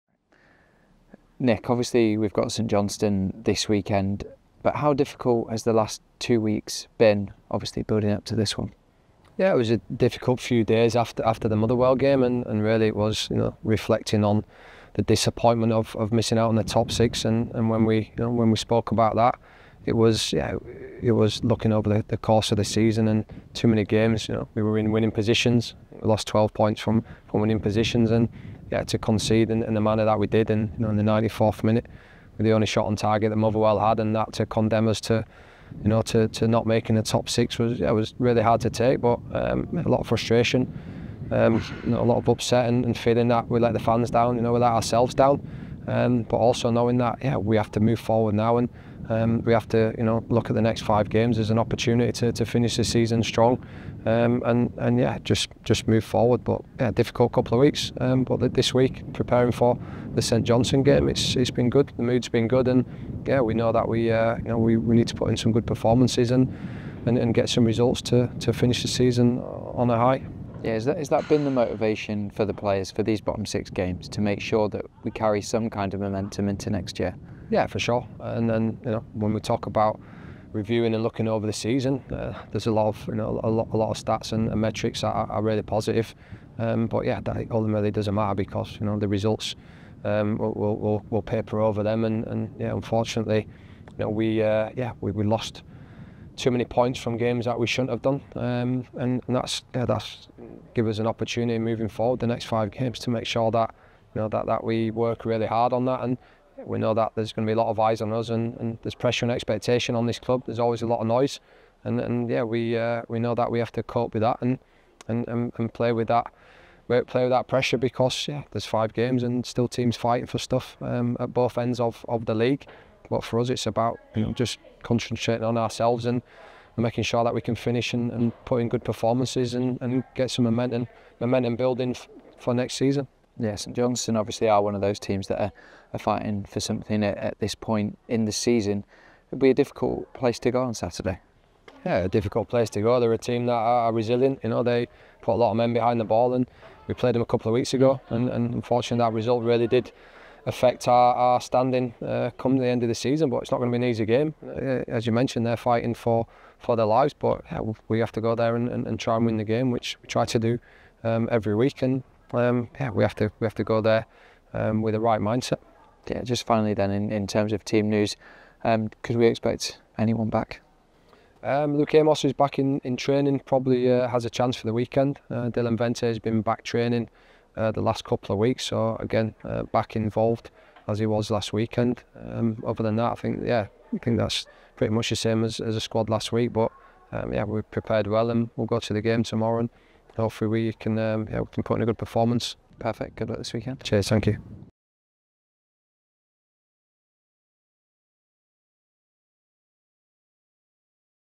St Johnstone vs Hibernian | Nick Montgomery's Preview | cinch Premiership
The Hibs Head coach previews tomorrow's league fixture away to St Johnstone.